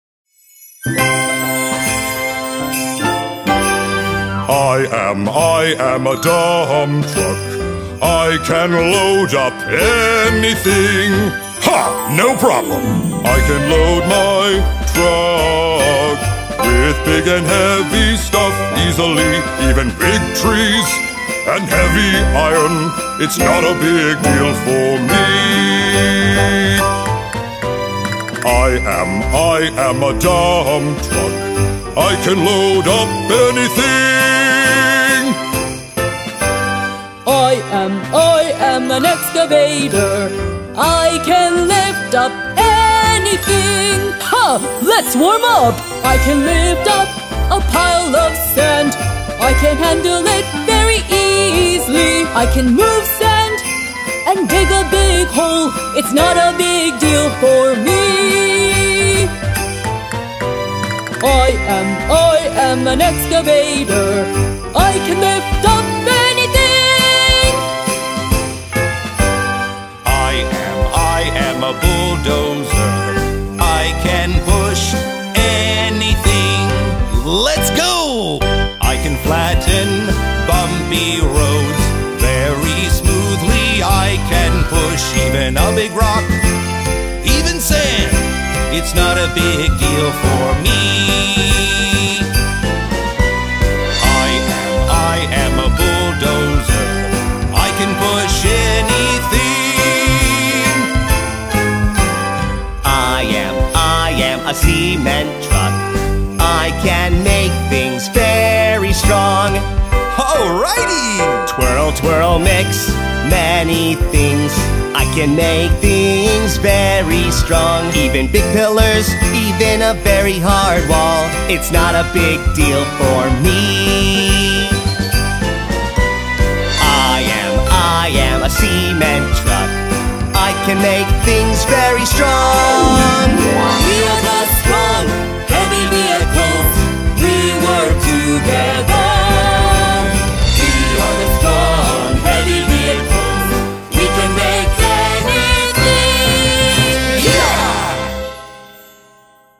The strong heavy vehicles l Tayo Toys Story l Tayo the Little Bus l Tayo Toy Play Show
Our strong heavy vehicles can sing and they sing well!! Bulldozer, excavator, dump truck, and cement truck! Do you guys know how they work? They work together on sites where heavy materials need to be mixed and loaded! What else do you guys know about heavy vehicles? It's the best song for kids and babies learn about vehicles.